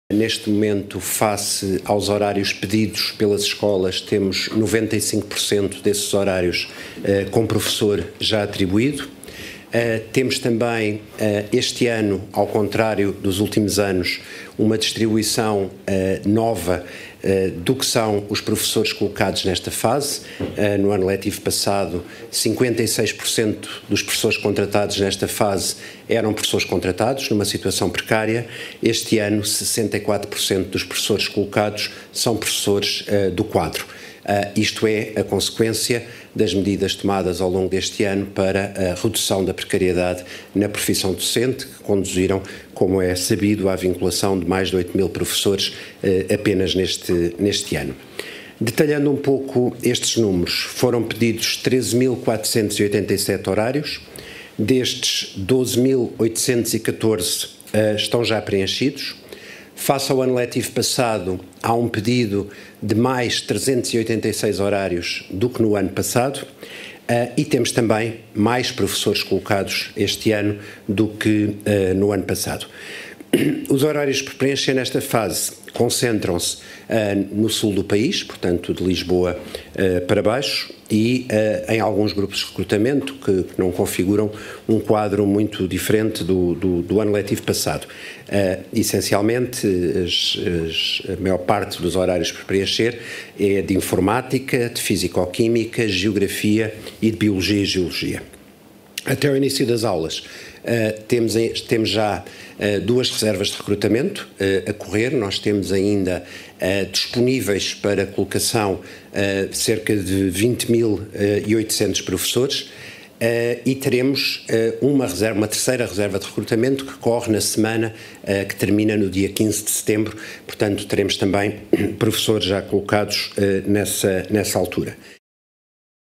Próximo ano letivo com 95% dos horários com professores já atribuídos
O próximo ano letivo tem 95% dos horários com professores já atribuídos, a maior parte do quadro, segundo dados hoje divulgados pelo Governo.